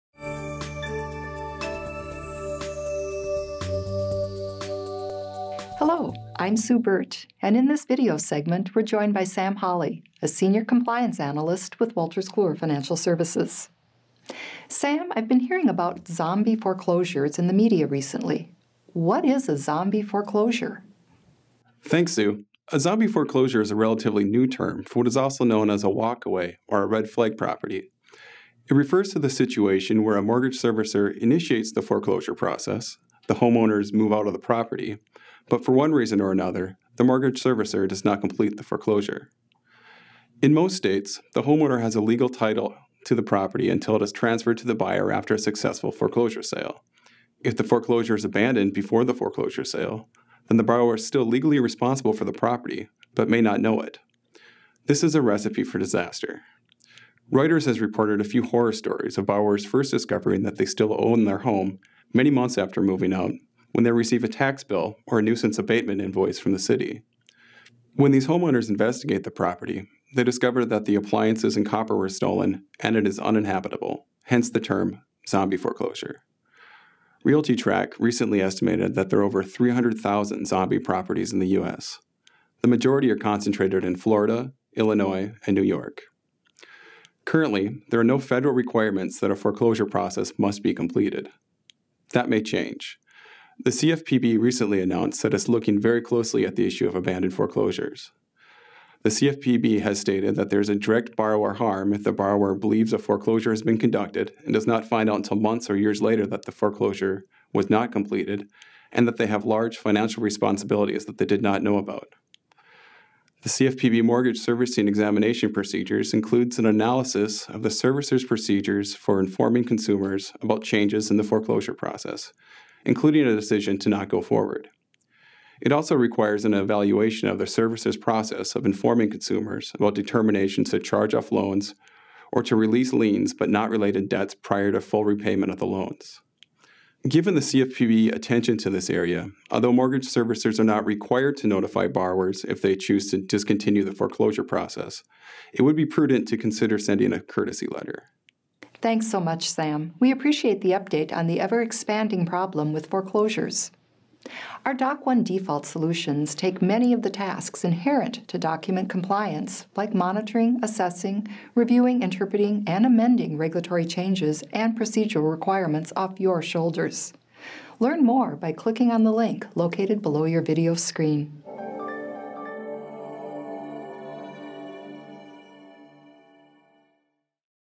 What is a Zombie Foreclosure?
Learn more about Zombie Foreclosures - Servicer initiates foreclosure process, homeowner moves out, but foreclosure never completed.